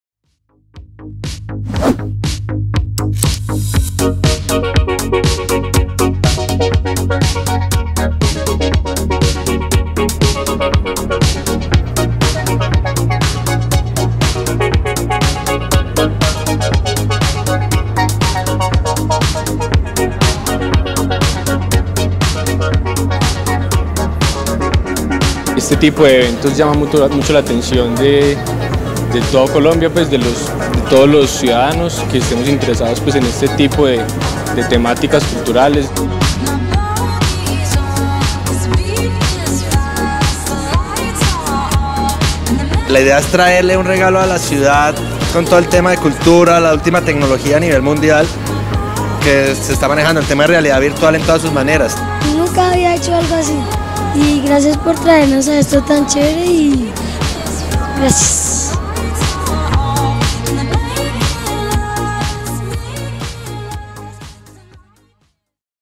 IndieBoX - IndieBo2015
El MinTIC presenta IndieBoX, lo último en Realidad Virtual e Interactividad para que Colombia esté a la vanguardia y se sincronice con esta nueva tecnología ya sea como observador o generador de contenido.

Cinco instalaciones llegan a America Latina por primera vez después de haber participado en los festivales de SUNDANCE y TRIBECA 2015.
Una producción de Curiosity y Colciencias en donde el público podrá experimentar la tecnología que está revolucionando los medios audiovisuales: gafas Oculus Rift, 3D, tabletas para interactuar con una película entre otros, serán encontrados en esta gran muestra de tecnología en IndieBo2015.

DIRECTOR 1 UNID
Catalina Ramírez Chaves

DIRECTOR 2 UNID
Christian Sarmanto

SOUND DIRECTOR
Juan Manuel Garcia

SOUND DESIGNER
Lina María Estevez

Alvaro J. Cavides
Sebastian Mendoza
Marcela Ávila
Miguel García
Juan David Vargas
Daniel Vesga
Nelson Vasquez

POST PRODUCTION
Catalina Ramírez Chaves

EDITION
Nelson Vásquez
Gabriela Zambrano
Julián Álvarez

HEAD OF COMMUNICATIONS
María Camila Pava

COMMUNICATION
David Montenegro
Diana Gomez
Laura Ardila
Andrés Jimenez
Andrés Briceño

Sara Goméz Ossa
Sandra Rojas

PHOTOGRAPHY COORDINATOR AND COMMUNICATIONS
Paula Andrea Angarita Coy

PHOTOGRAPHY
Miguel García
Jonas Ratziunas
Federico Reyes
Carlos F. Congote
Daniel Felipe Ochoa
Andrés Felipe Mateus
Angie Moreno